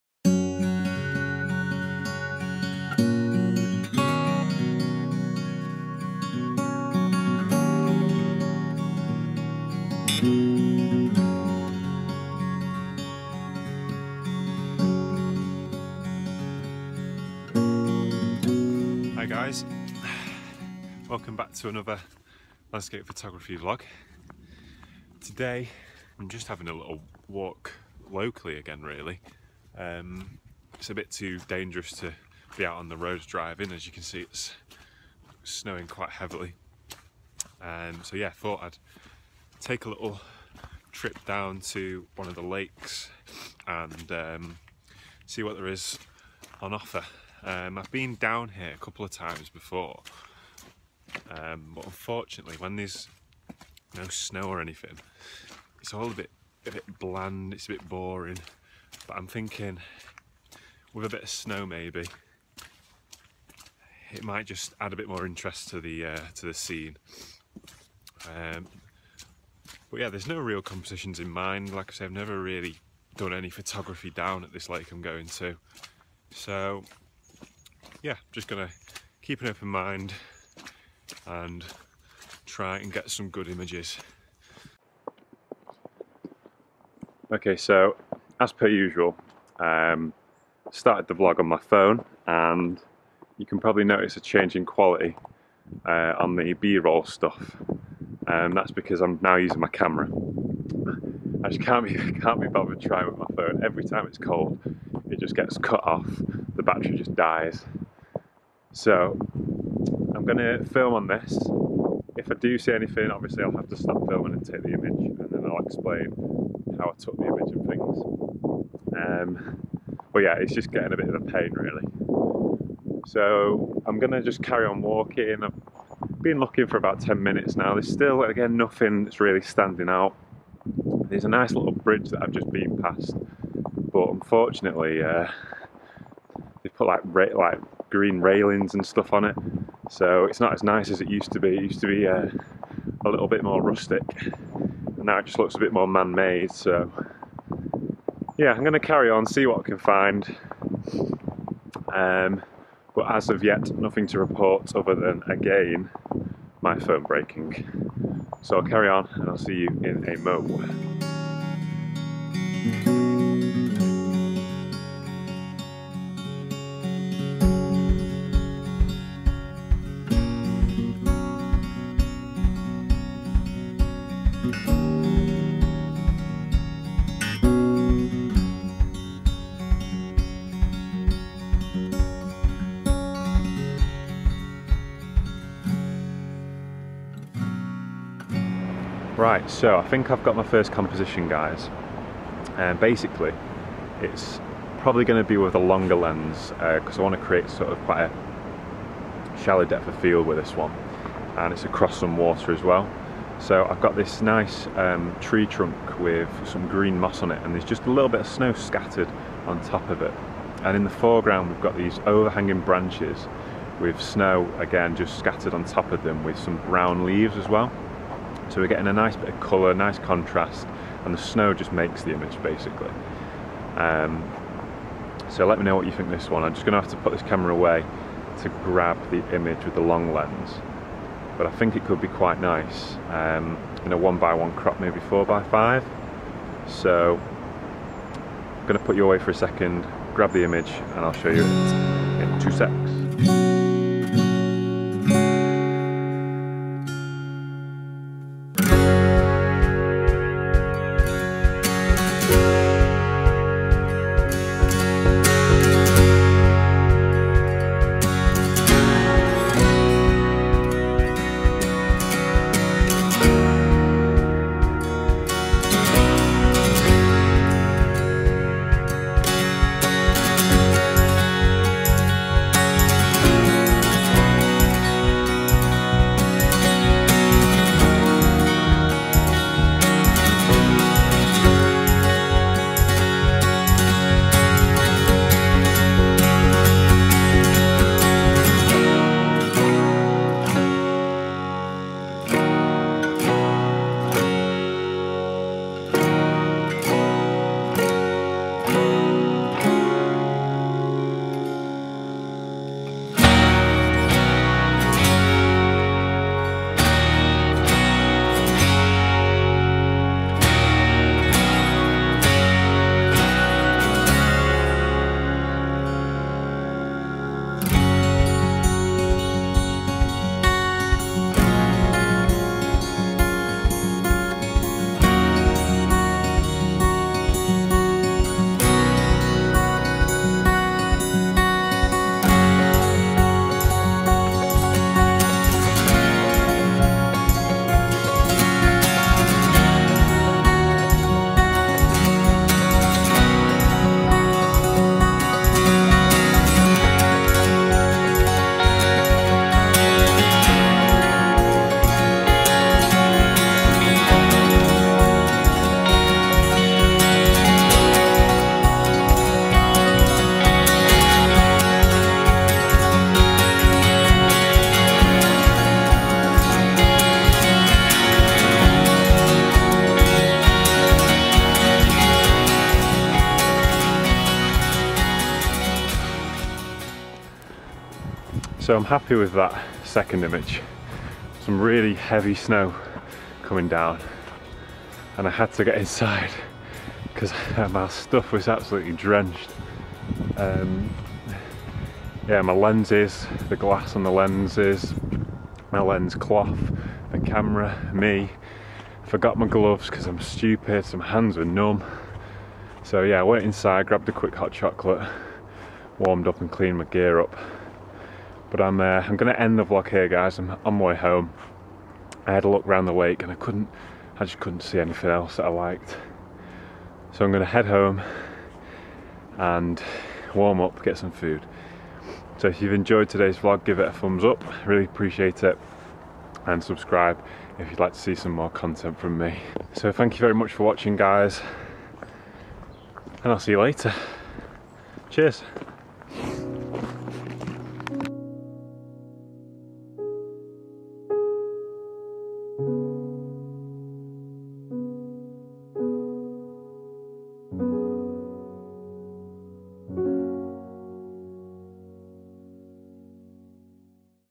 Landscape Photography | Snow, Snow and a peacock?
The first snow of the year (for me) and wow did it come down heavy at one point! A spare few hours out with the camera and a couple of images to be had.

Let me know if you prefer the vlog with my camera or like in the other videos with the phone camera.